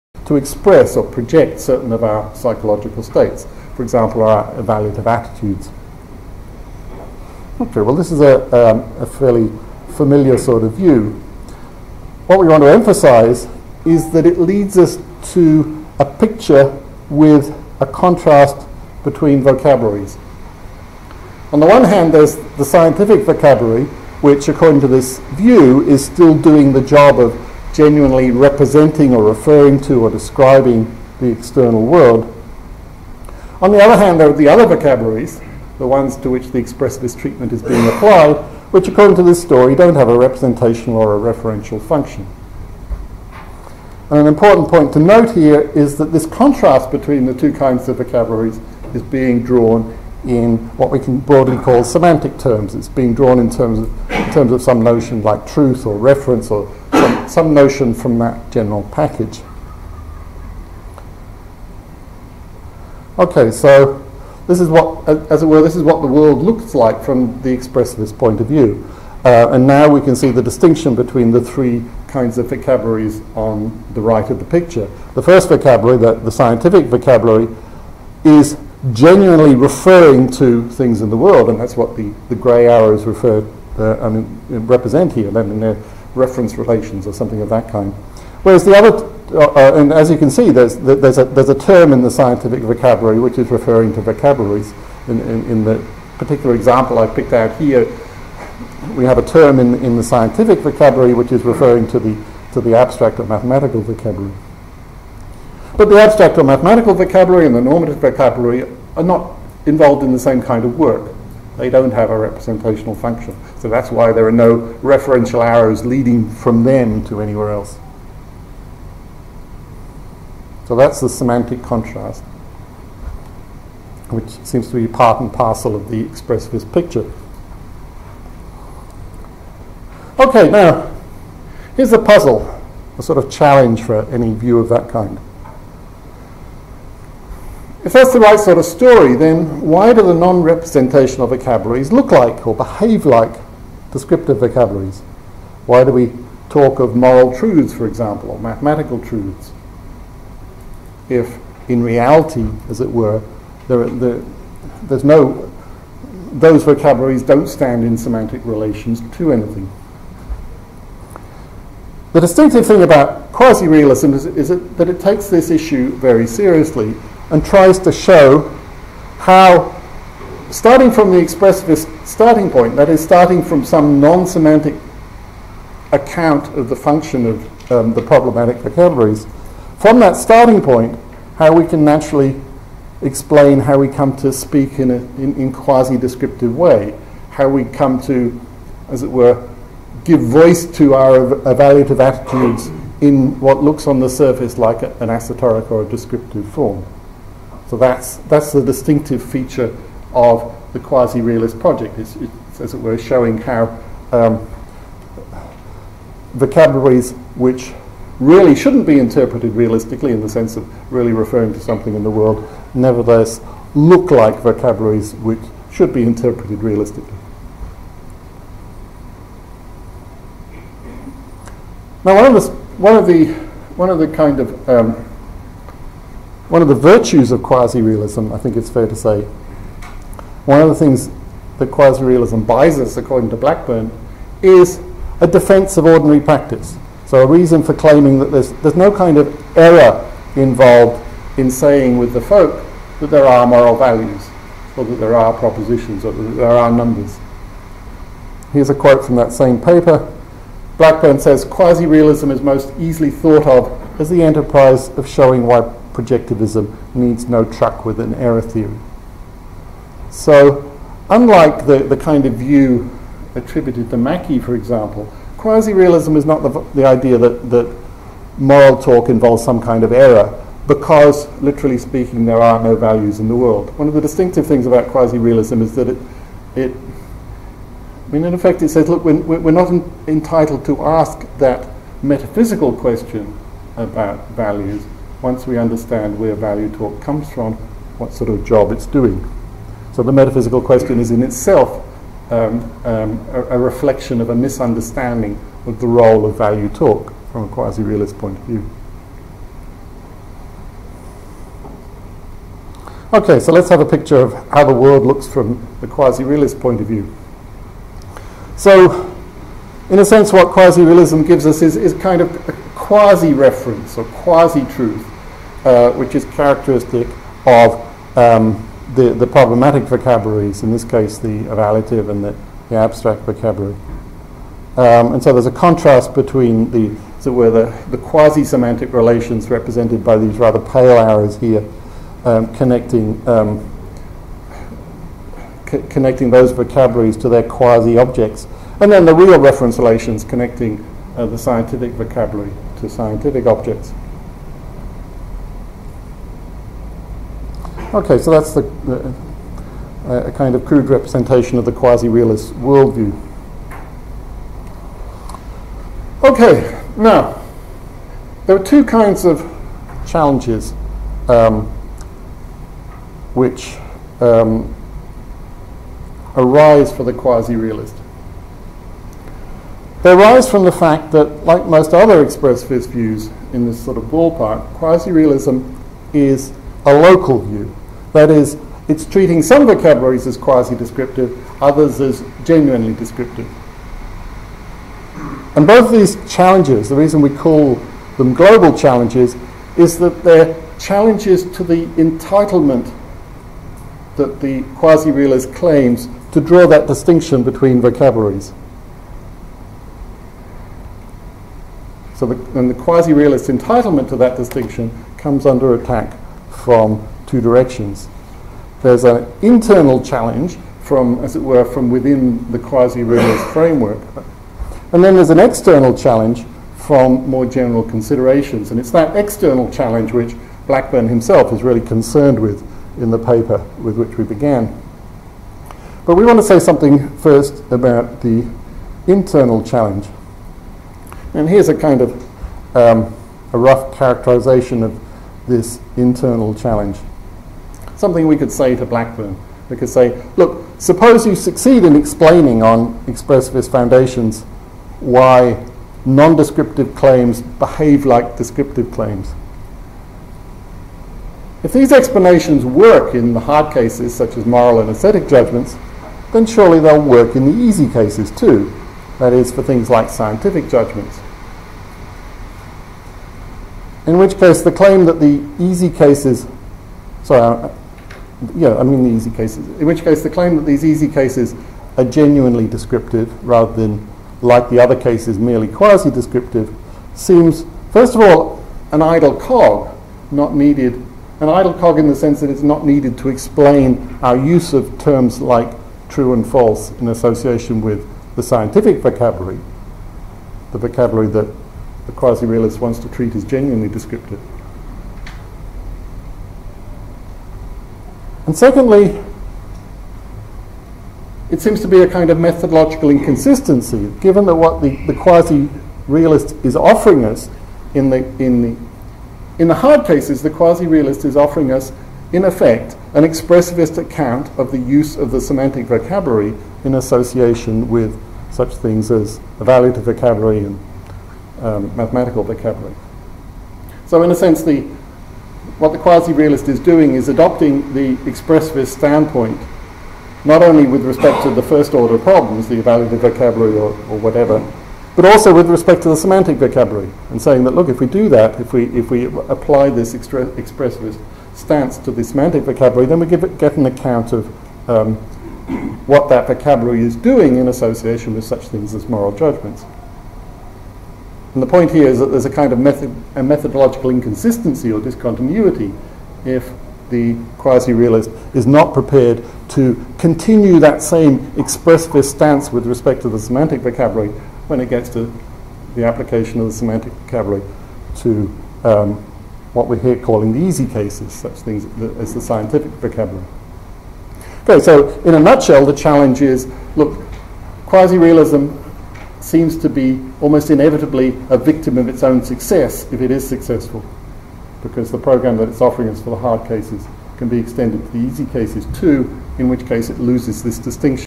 Huw Price: Pragmatism, Expressivism and the Global Challenge Part 2
Pragmatism, Expressivism and the Global Challenge
Huw Price & David Macarthur (Sydney)

Expressivism is typically a local view. Thus an expressivist about moral or aesthetic judgments will contrast these judgments to genuinely descriptive claims (such as those of science, perhaps). This contrast comes under pressure from several directions, however.

• Externally, it has been thought to be threatened by minimalism about truth, which—it is argued— leaves no room for the view that ethical claims are not really truth-evaluable. (If truth is thin, then it seems easy for moral claims be truth-evaluable: it is sufficient that X is good is true iff X is good, and who disputes that?)

• Internally, it seems threatened by the quasi-realist program of explaining on expressivist foundations why non-descriptive claims behave like descriptive claims. If these explanations work in the hard cases, such as moral and aesthetic judgements, then surely theyll work in the easy cases, too—in which case the idea that the easy cases are genuinely descriptive seems an idle cog, not needed to explain the use of the statements in question.

As first sight, it may seem as if these pressures push in opposite directions. Doesnt the first threaten to make everything descriptive, and the second to make everything expressive? So a problem for a local expressivist either way, in other words, but a very different kind of problem, in each case.

On closer inspection, however, it turns out that both pressures push in the same direction, towards global expressivism. Moreover, contrary to some claims, the resulting position is not a homogeneous or quietist view of language, unable to make the substantial claims that expressivists wanted to make about the function of particular domains of discourse. What is lost is simply the idea that there is a substantial descriptive or representational function, characteristic of some domains but not others.

In this paper we explore these ideas against the background of some remarks by Simon Blackburn, and relate the discussion to a disagreement between Brandom and Rorty.

The paper on which this talk is based can be read